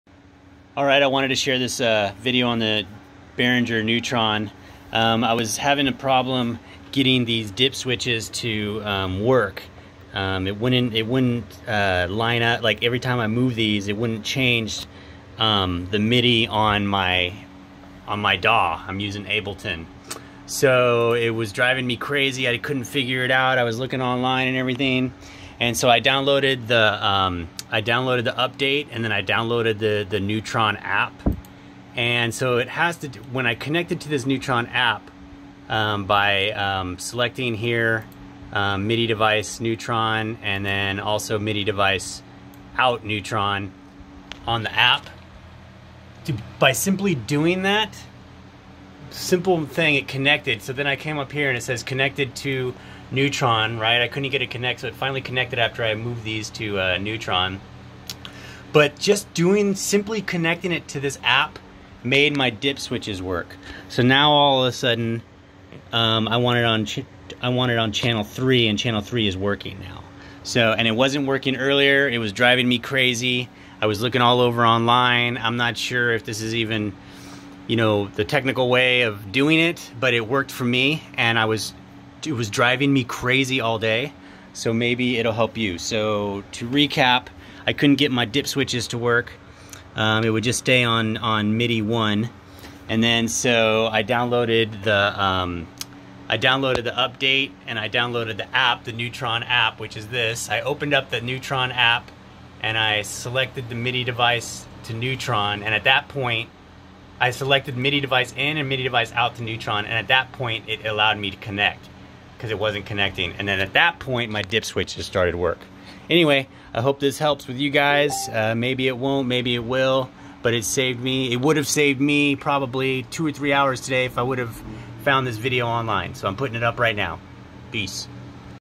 NEUTRON BEHRINGER DIP SWITCHES AND APP NOT WORKING [FIX]
If you're dip switches aren't working, or you can't get the Neutron App to work then this video is for you.